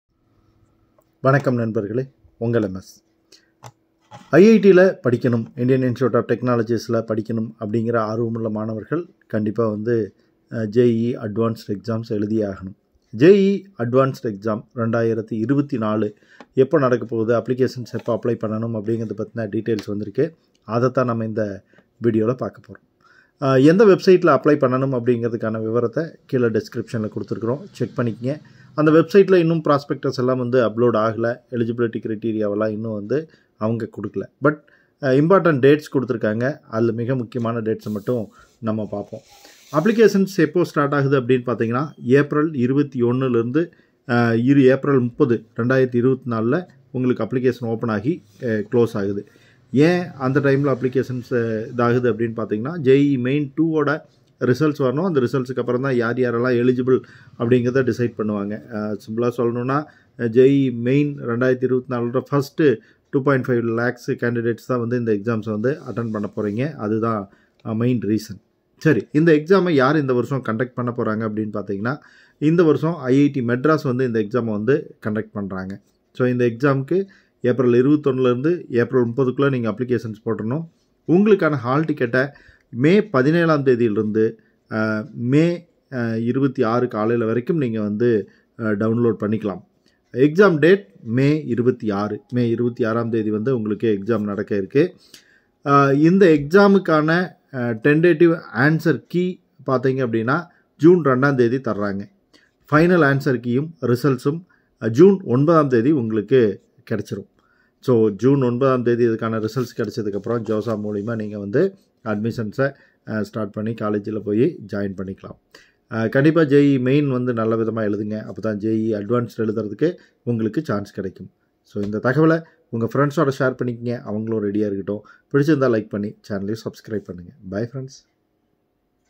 JEE Advanced 2024 | Application & Exam Date detail | When can I download JEE advanced Hall Ticket?
JEE Advanced 2024 | Application & Exam Date detail | When can I download the JEE Advanced Hall Ticket?